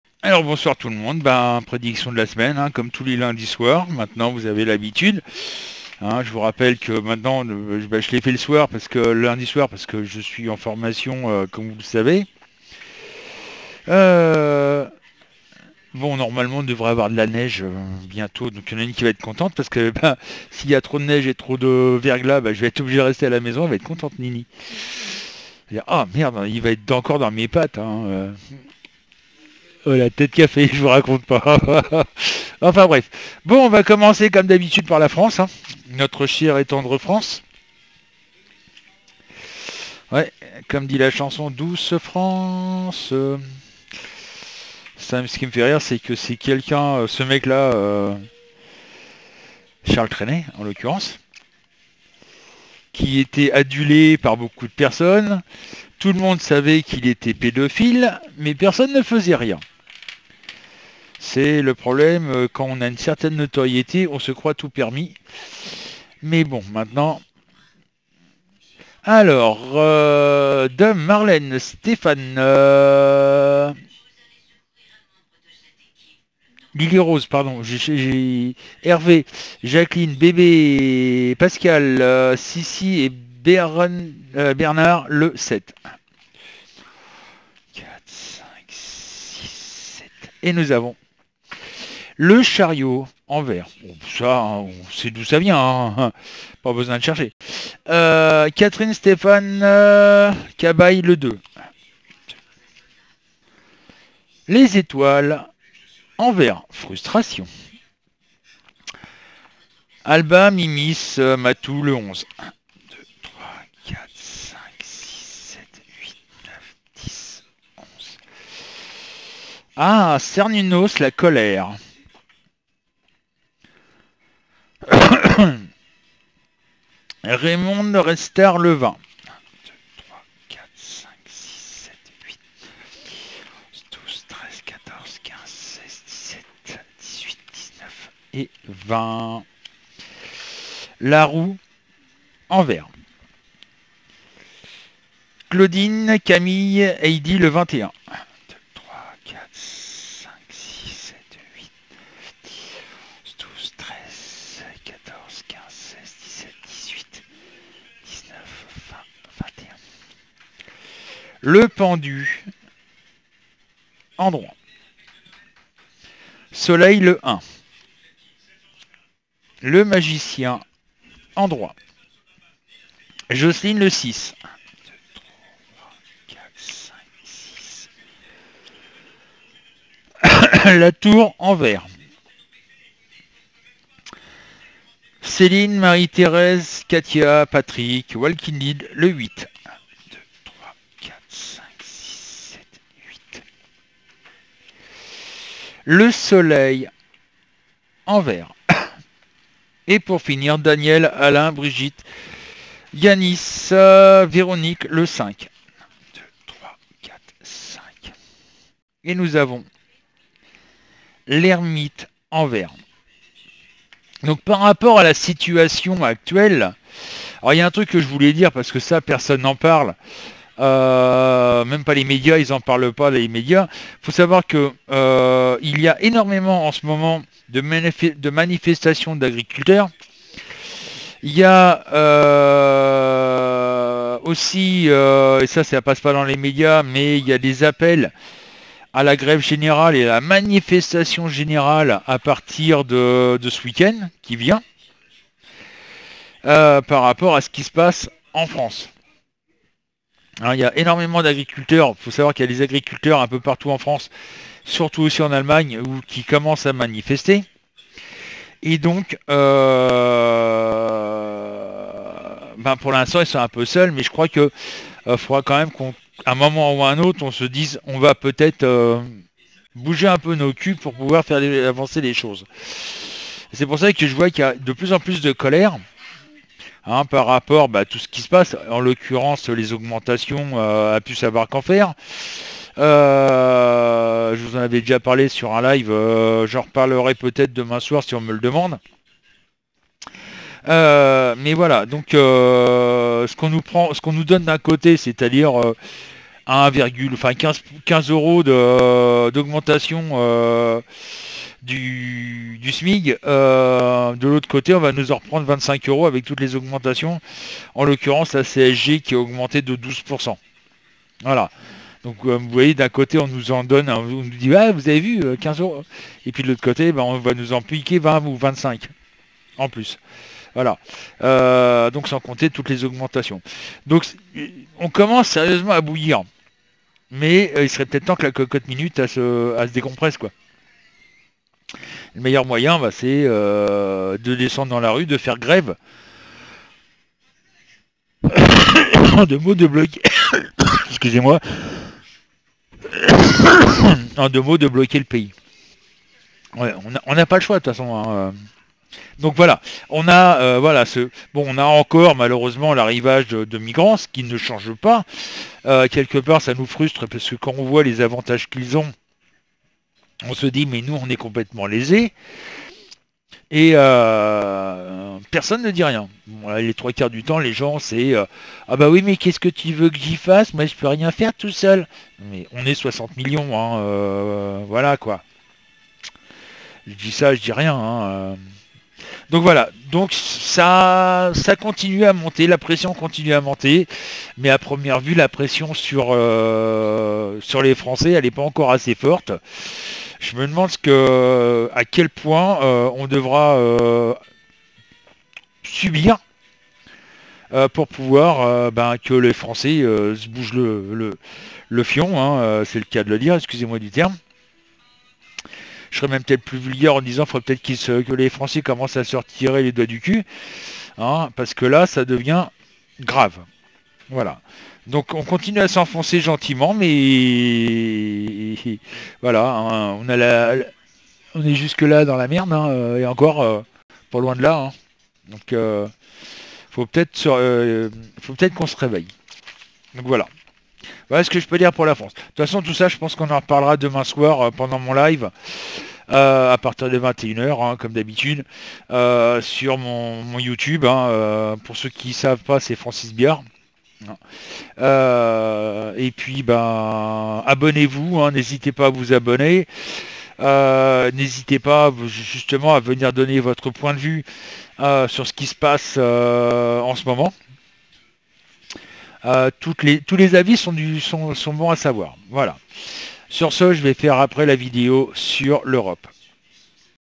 prédiction France du 15/01
bonsoir , en ce lundi 15 janvier , je viens vous présenter mes prédictions pour la France